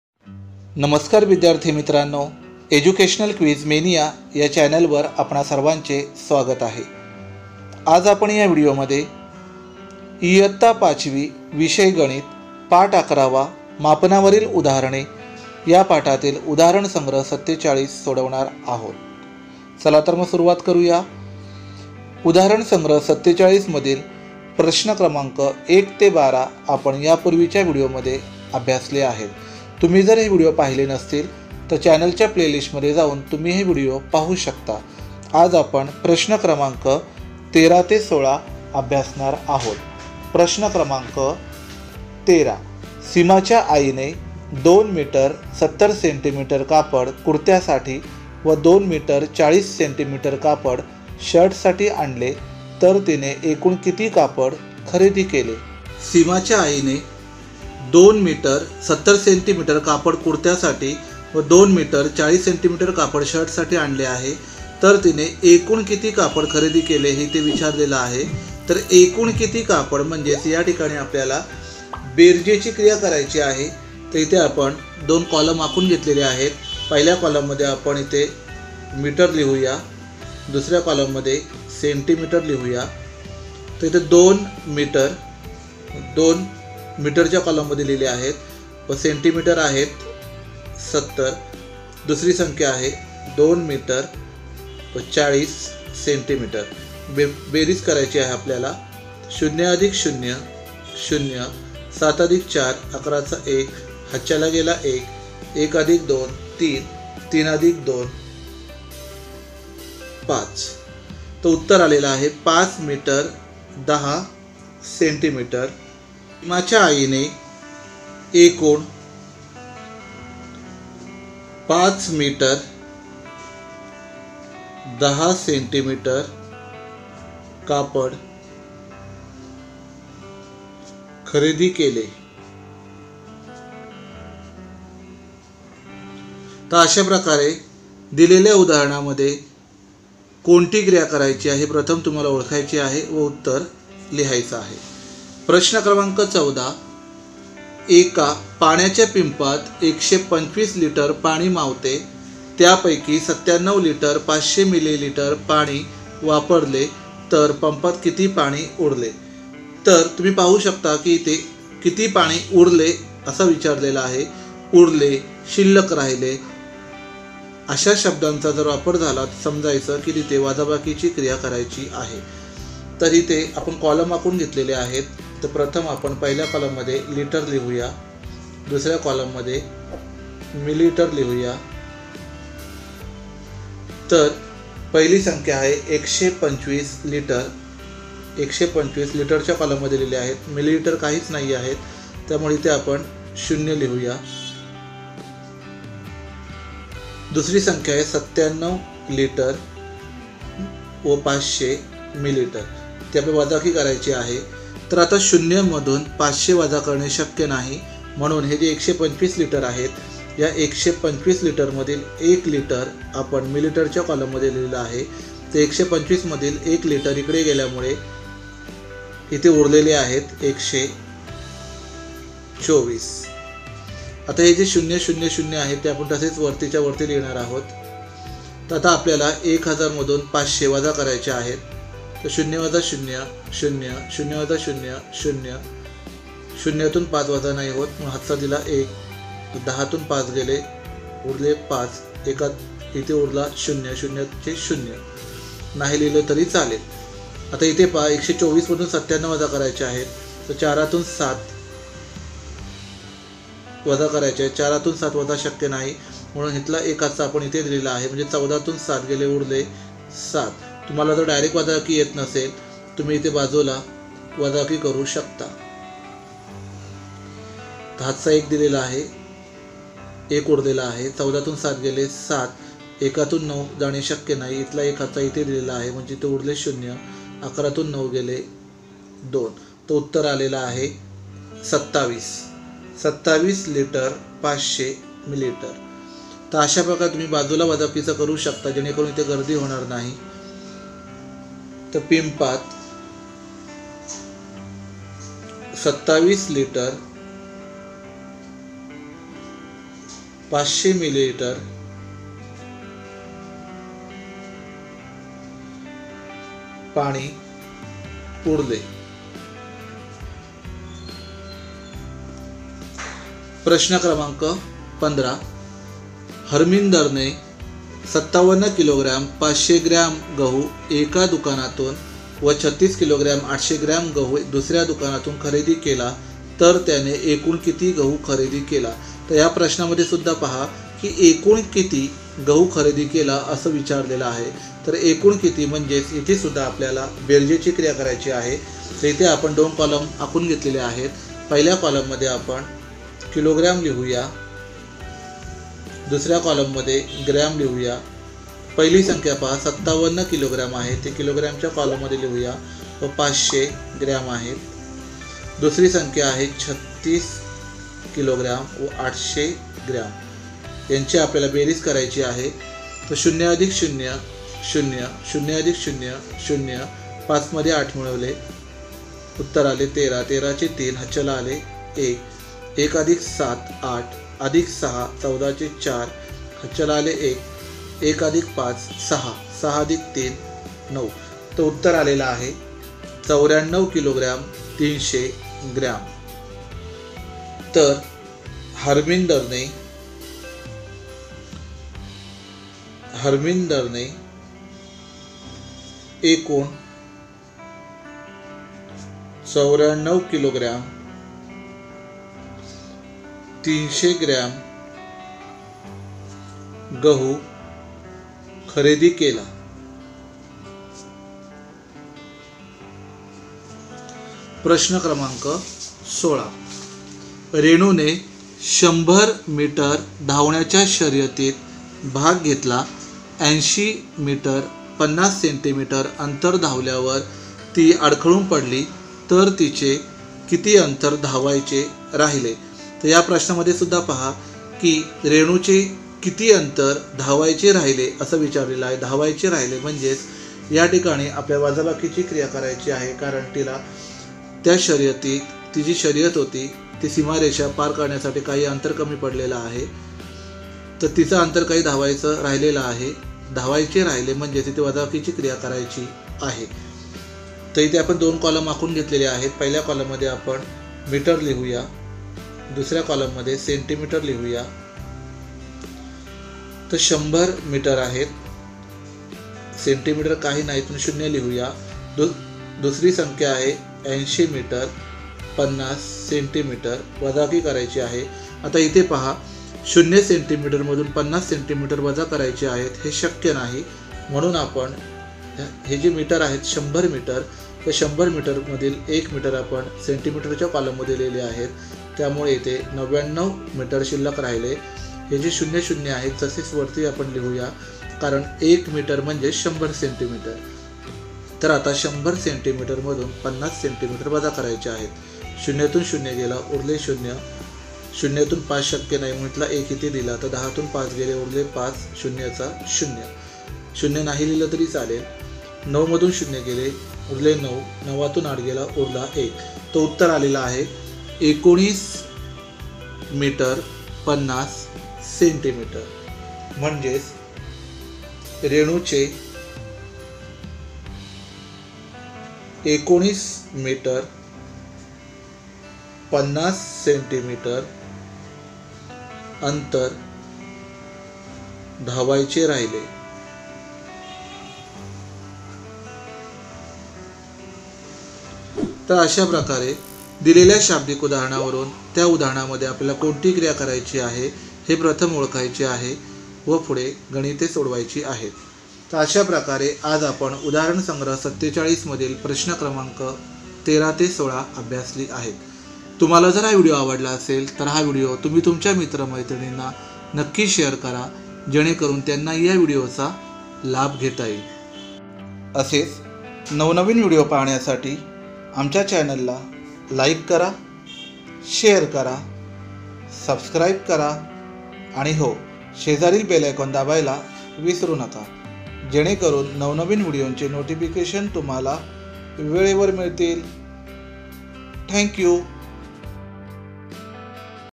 उदाहरणसंग्रह 47 पाचवी गणित| मापनावरील उदाहरणे| Udaharan sangrah 47 class 5 | Problem set 47 pachavi
विद्यार्थ्यांनो या व्हिडिओ मध्ये आपण इयत्ता-पाचवी , विषय-गणित , 11. मापनावरील उदाहरणे  या पाठातील उदाहरणसंग्रह 47 प्रश्न क्र. 13, 14, 15, 16 सोडवणार आहोत.